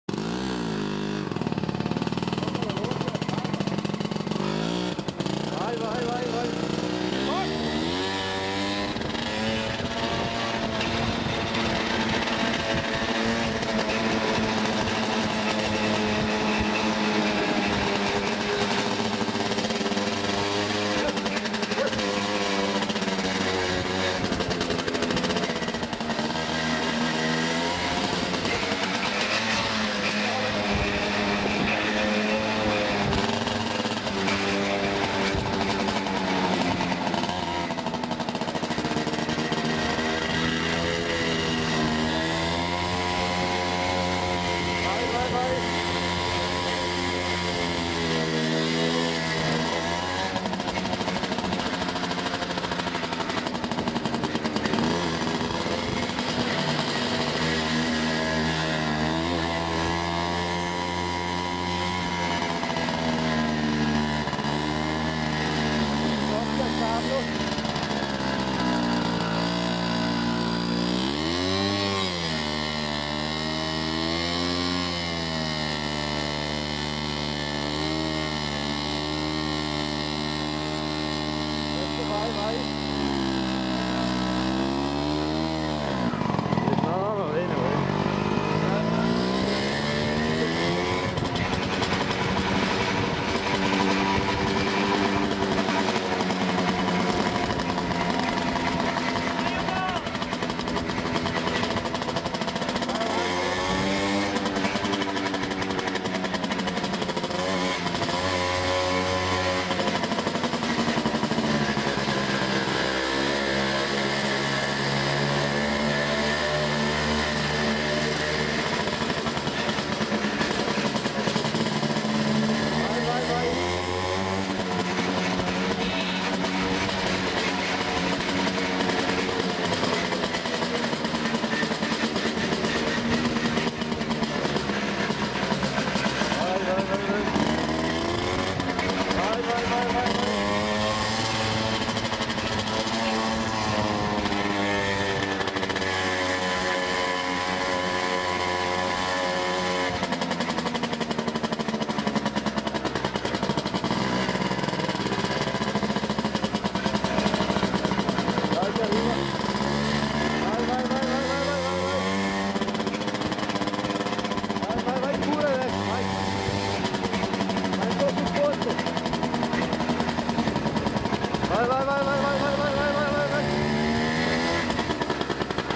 NUOVO RECORD DEL MONDO DEL "GIRO DELLA GLERA" - PICCIOLI UGO - 2'55''50 - 13/10/2013
Ecco il Video del  RECORD DEL MONDO del  "GIRO DELLA GLERA" (Adrara S.Rocco -- Prov.Bergamo) migliorato  in 2'55''50 sempre da  Piccioli Ugo il 13 ottobre 2013